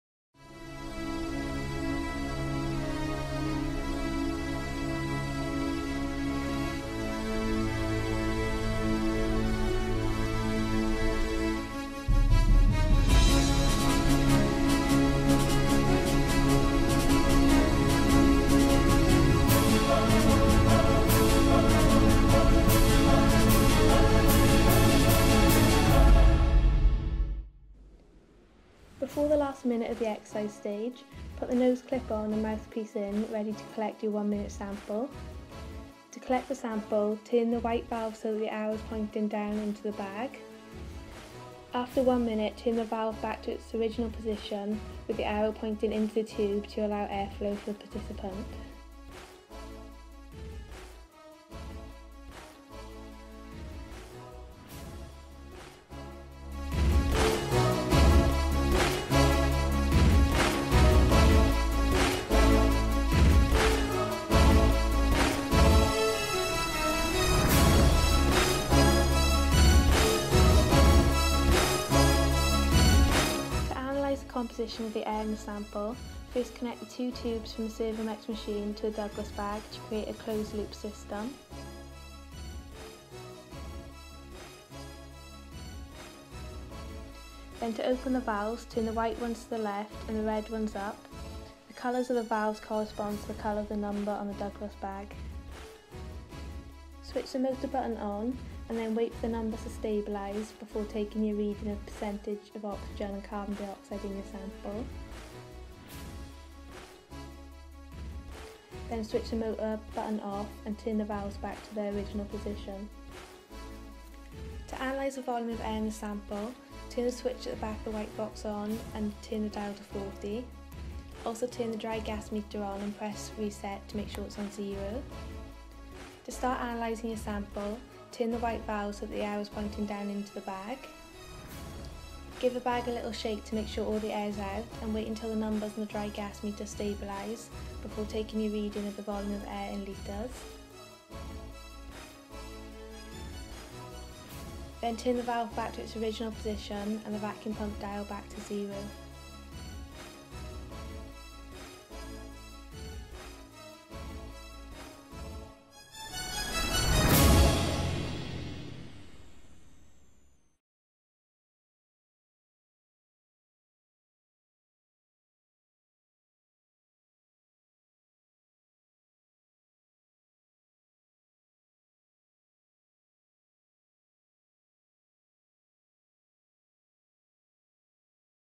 Ventilatory Threshold using Douglas Bags
Example of how to use the Douglas bags for determining ventilatory threshold. Video demonstrates how to analyse the sample.